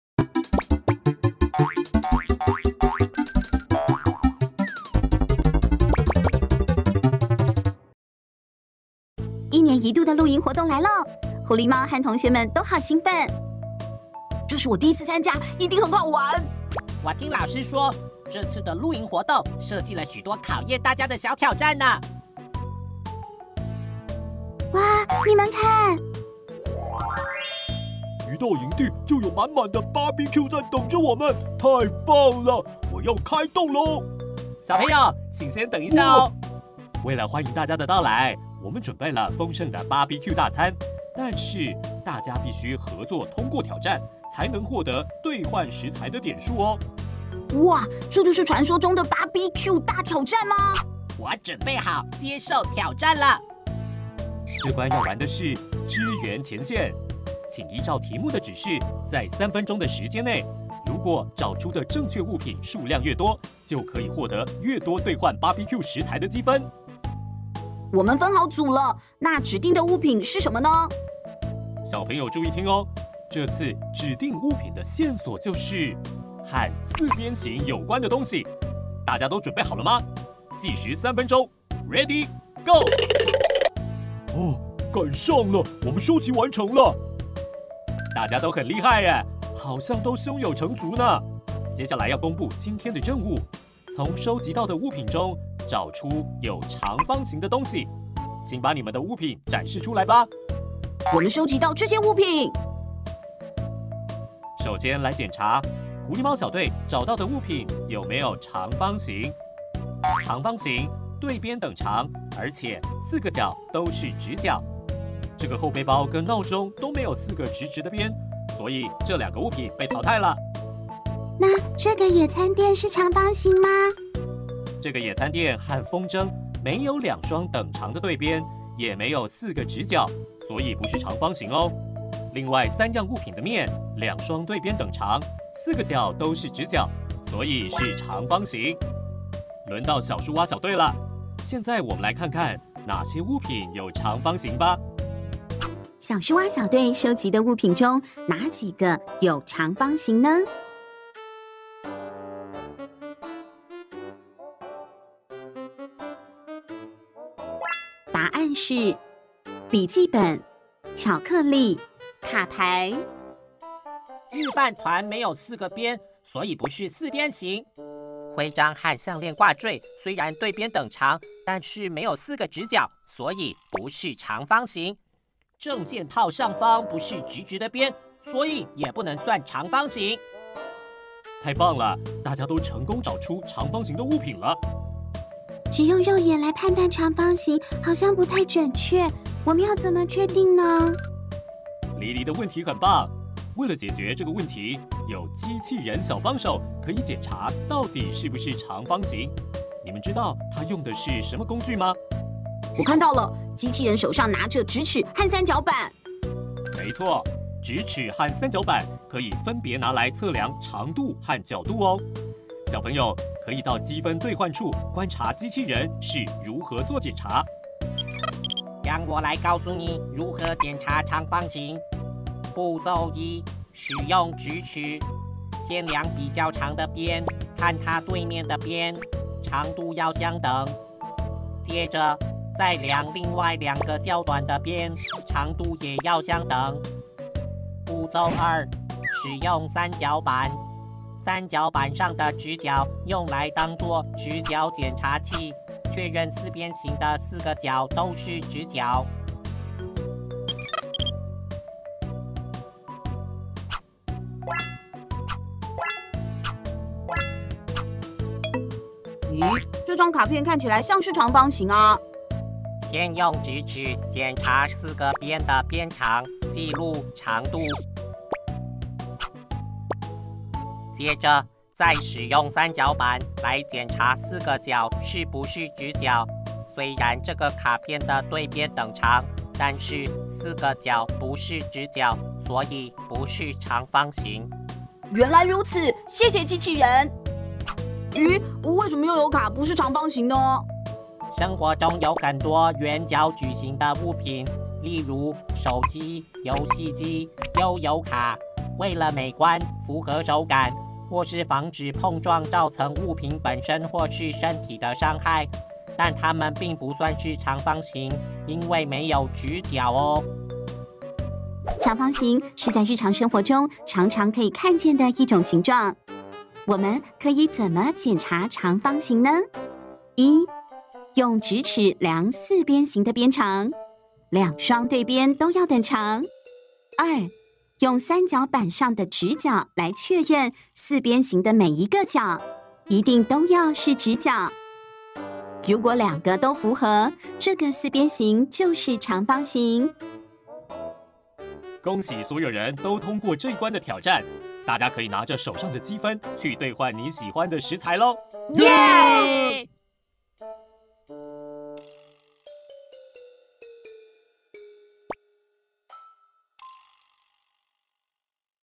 108新課綱｜小三數學｜【解題】使用工具判斷長方形
你會使用工具判斷圖形是否為長方形嗎？

0:00 引起動機、情境一
2:25 練習 1
2:37 練習 1 解答
3:01 情境二
5:11 總結

🦥 速度太快？調整播放速度為0.75倍速，放慢腳步一起學習！
👀 影片含CC字幕，觀看時歡迎開啟。